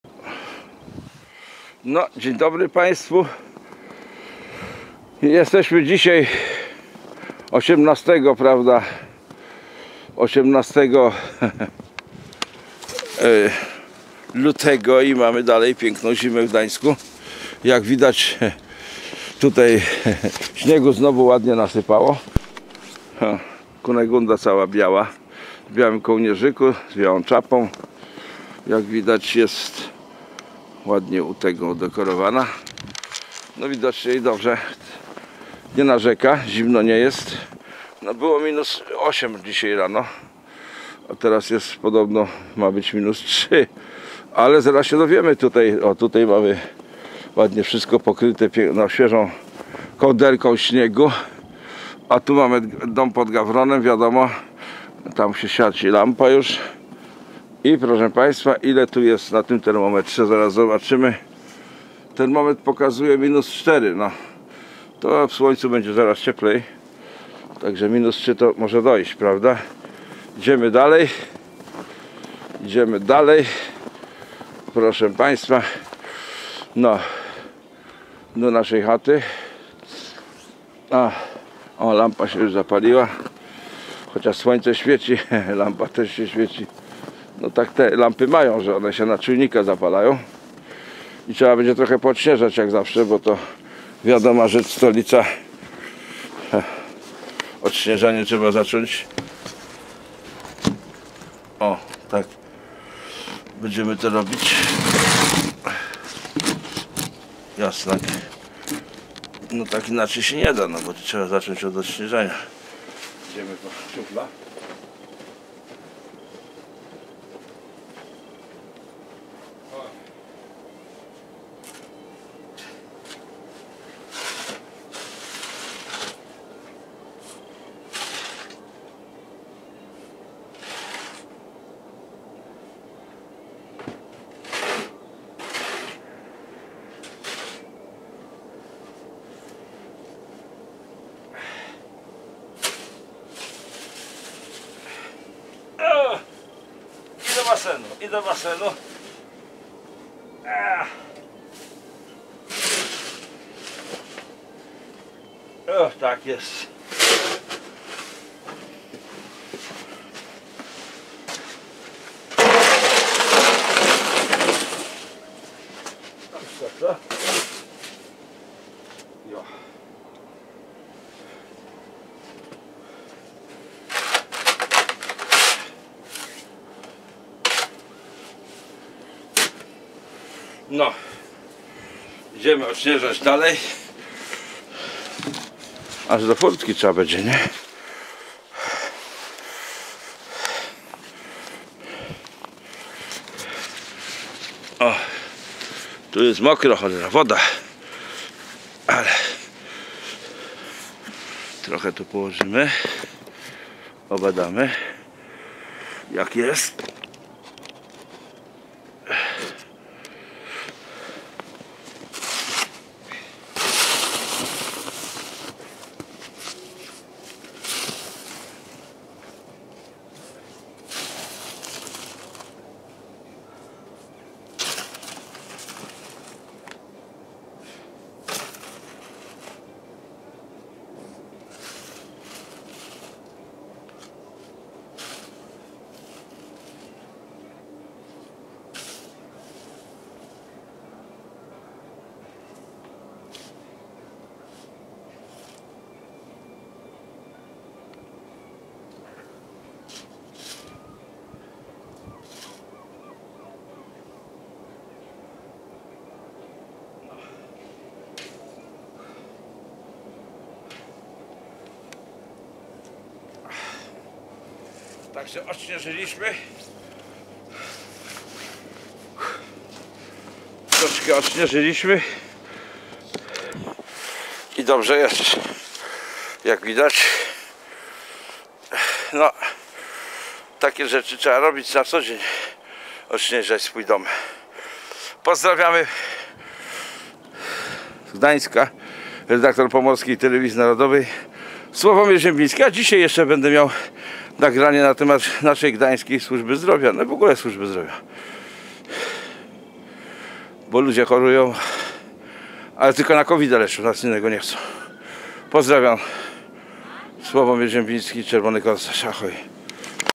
CODZIENNE ODSNIEŻANIE DOMU JEST KONIECZNE, ZA NIEODŚNIEŻENIE CHODNIKA OBOK DOMU GROZI NAM MANDAT!
Każdego dnia Korsarz bierze szufle do śniegu i usuwa ze ścieżki śnieg,  aby można byłą suchą noga wejść do nowego domu,  gdzie mamy Studio P TV N. Następnie czyści ze śniegu buty i wchodzi do domu ,  aby tam nagrać swoje wypowiedzi .Dziś opowiedział ,  jak to działa Służba Zdrowia i czego możemy oczekiwać po lekarzach? Ponadto zaapelował do Mateusza Morawieckiego Premiera RP aby uwolnił Polskie Firmy od Lockdaunu i obostrzeń ,  których żąda znowu minister Niedzielski!?

Pomorska TV NARODOWA
red. Sławomir Ziembiński